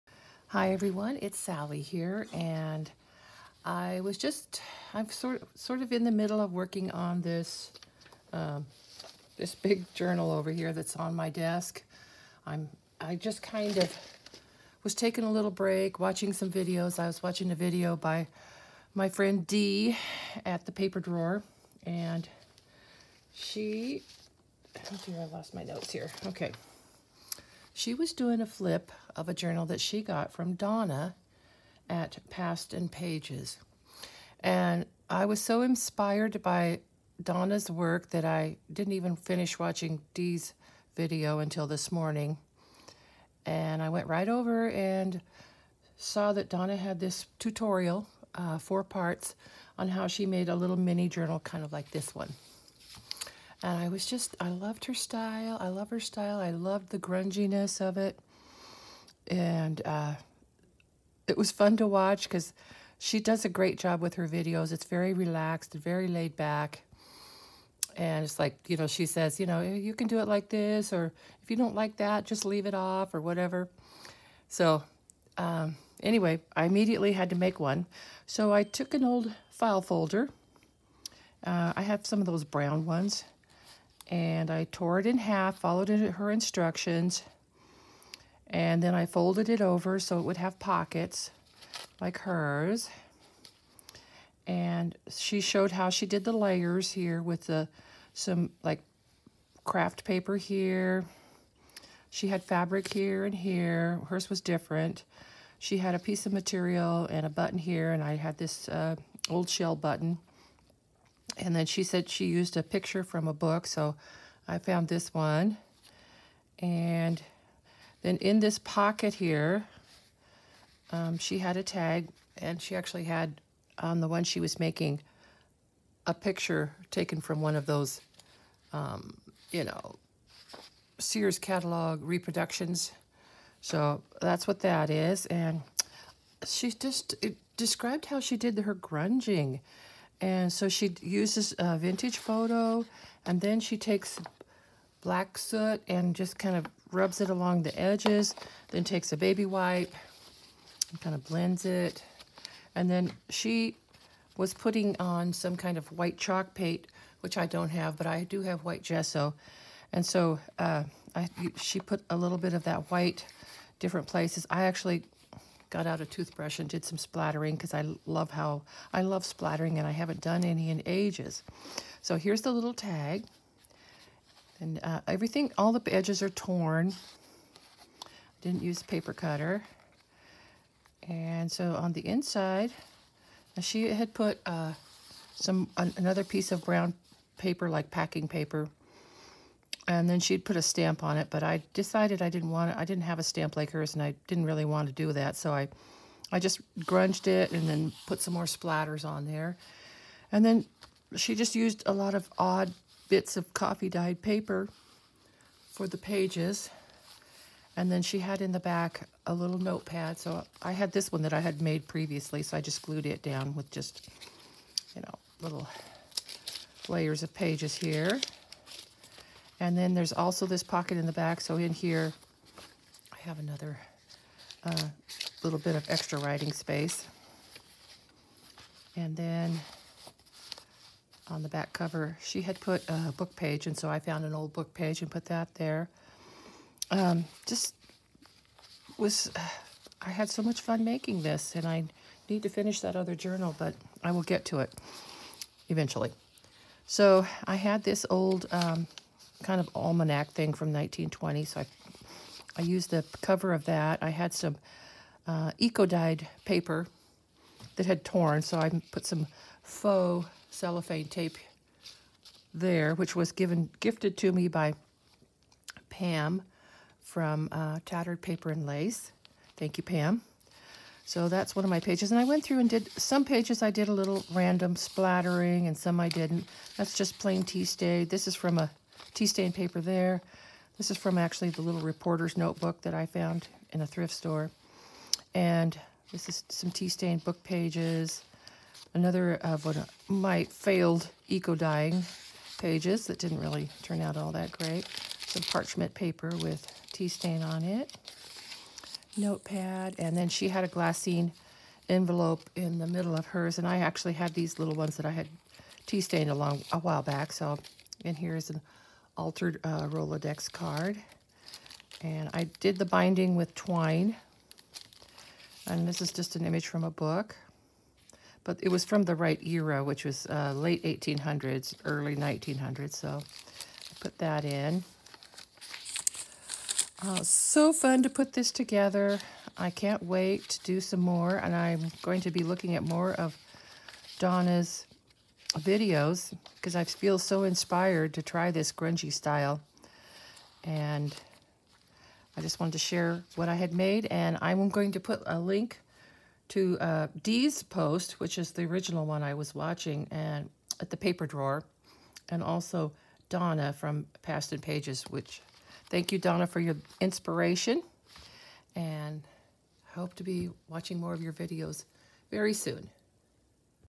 Grungy little journal
I saw a flip of a grungy journal that really inspired me to try that style. Dee from The Paper Drawer showed it in a journal share. Donna from Past and Pages created the journal.